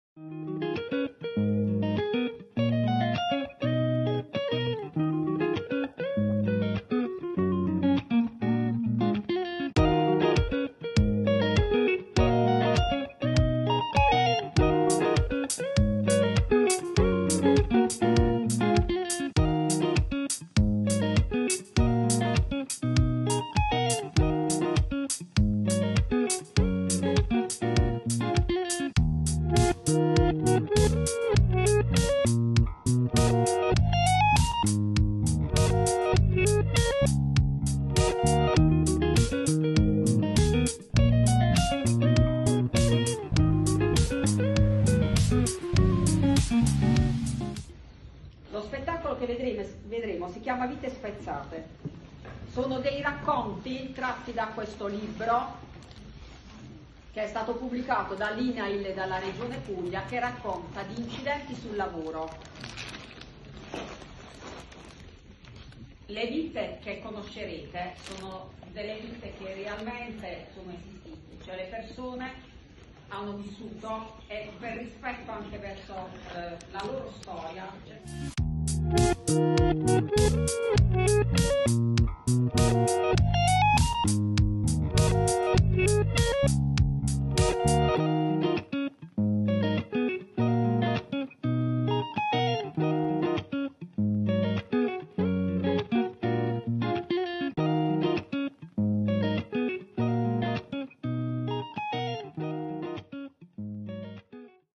VITE SPEZZATE - DIREZIONE REGIONALE PUGLIA
Fonte: Itet "Dante Alighieri"

Il video relativo alla partecipazione dell'Istituto tecnico economico e tecnologico "Dante Alighieri" di Cerignola (Fg)